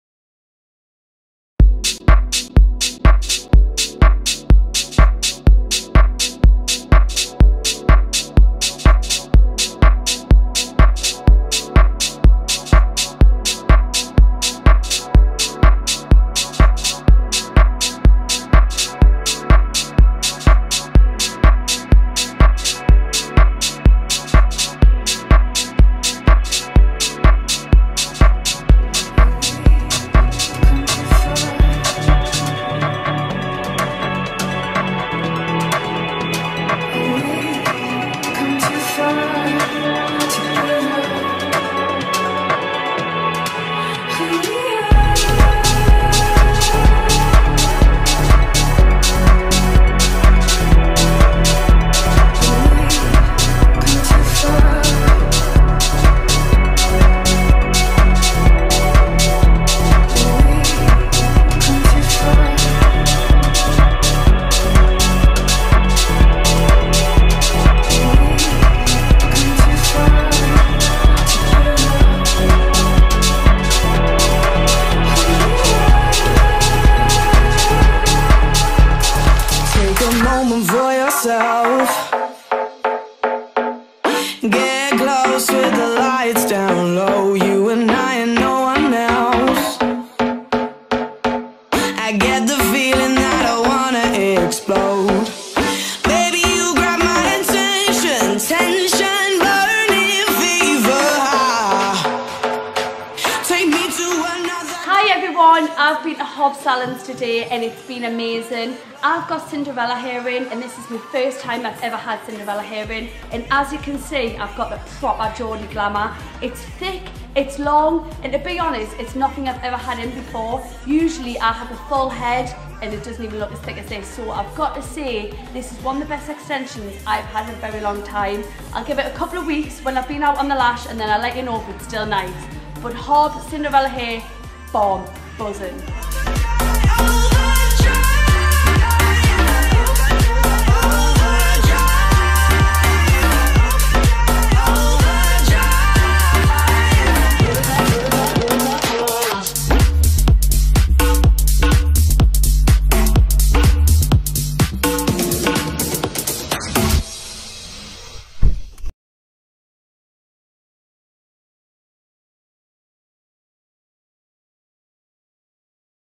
Cinderella Hair & HOB Salons Present Geordie Shore Beauty Sophie Kasaei
On Tuesday 5th August 2014 Cinderella Hair & HOB Salons teamed up with the Geordie Shore Beauty Sophie Kasaei!

Our video will show everything from how Cinderella Hair Extensions are applied, including our system through to Sophie's own personal review of both Cinderella Hair & HOB Salons!

Sophie had 22inch Body Wave colour 8 applied, to create a long, thick & full of life look!

For all things Cinderella Hair have a look at our

For all information regarding HOB Salons take a look at their